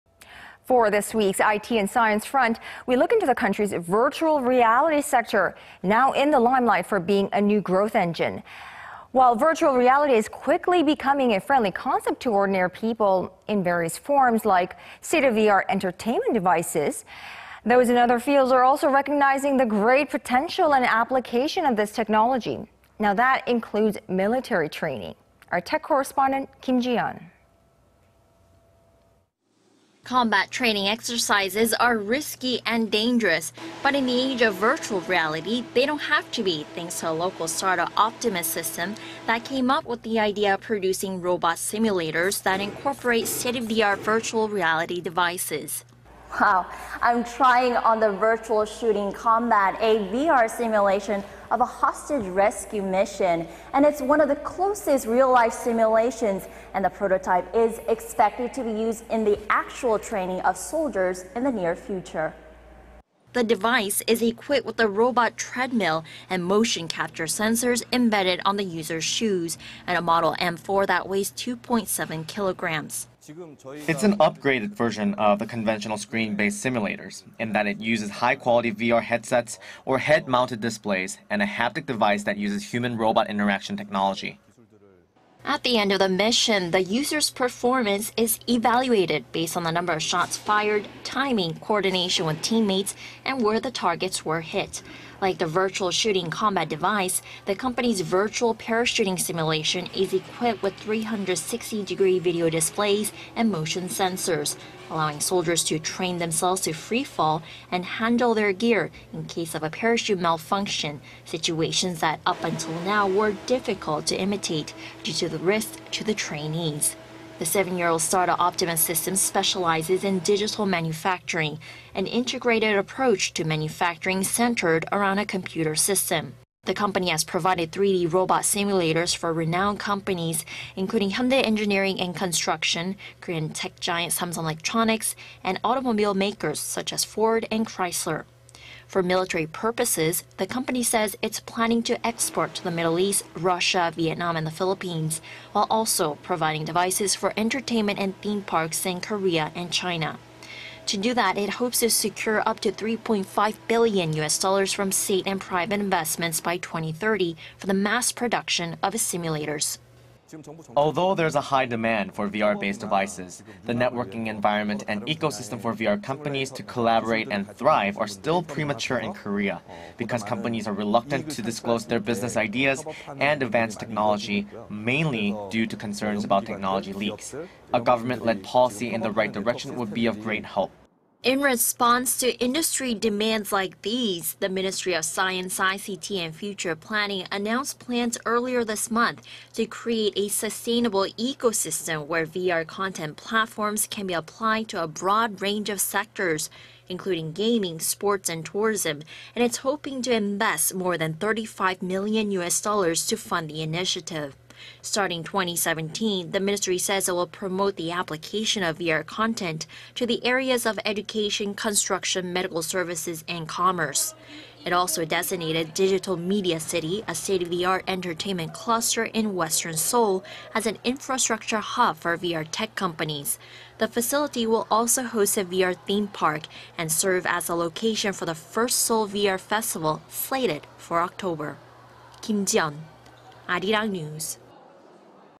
Korean startup releases VR simulators for military training
VR 기반 군사훈련용 로봇 시뮬레이터 나왔다
  For this week's IT and Science Front... we look into the country's virtual reality sector... which is in the limelight for being a new growth engine.
While virtual reality is quickly becoming a friendly concept to ordinary people in various forms - state of the art entertainment device would be one - those in other fields are also recognizing the great potential and application of this technology.
THAT includes military training.
Our tech correspondent Kim Jiyeon.
  Combat training exercises are risky and dangerous.
But in the age of virtual reality, they don't have to be -- thanks to a local startup that came up with the idea of producing robot simulators that incorporate state-of-the-art virtual reality devices.

   "I'm about to try on the 'virtual shooting combat,' a VR simulation of a military training exercise in a hostage rescue situation. It's one of the closest real life simulations and the prototype is expected to be used in the actual training of soldiers."

  The device is equipped with a robot treadmill and motion-capture sensors embedded on the users' shoes and a model M4 that weighs 2-point-7 kilograms.

    "It's an upgraded version of the conventional screen-based simulators... in that it uses high-quality VR headsets, or head-mounted displays, and a haptic device that uses human-robot interaction technology."

  At the end of the "mission," the users' performance is evaluated... based on the number of shots fired, timing, coordination with teammates... and where the targets were hit.
Like the "virtual shooting combat" device, the company's "virtual parachuting" simulation is equipped with 360-degree video displays and motion sensors,... allowing soldiers to train themselves to free fall and handle their gear in case of a parachute malfunction -- situations that up until now were difficult to imitate due to the risks to the trainees.

  The seven-year-old startup Optimus System specializes in digital manufacturing,... an integrated approach to manufacturing centered around a computer system.
The company has provided 3D robot simulators for renowned companies including Hyundai Engineering and Construction, Korean tech giant Samsung Electronics and automobile makers such as Ford and Chrysler.
  For military purposes, the company says it's planning to export to the Middle East, Russia, Vietnam and the Philippines,... while also providing devices for entertainment and theme parks in Korea and China.
To do that, it hopes to secure up to 3-point-5-billion U.S. dollars from state and private investments by 2030 for the mass production of its simulators.

    "Although there's a high demand for VR-based devices... the networking environment and ecosystem for VR companies to collaborate and thrive are still premature in Korea... because companies are reluctant to disclose their business ideas and advanced technology,... mainly due to concerns about technology leaks. A government-led policy in the right direction would be of great help."

  In response to industry demands like these, the Ministry of Science, ICT and Future Planning announced plans earlier this month to create a sustainable ecosystem where VR content platforms can be applied to a broad range of sectors...  including gaming, sports and tourism.
And it's hoping to invest more than 35-million U.S. dollars to fund the initiative.
Starting 2017, the ministry says it will promote the application of VR content to the areas of education, construction, medical services and commerce.
It has also designated Digital Media City, a state-of-the-art entertainment cluster in western Seoul,... as an infrastructure hub for VR tech companies.
The facility will also host a VR theme park and serve as the location for the first Seoul VR Festival, slated for October.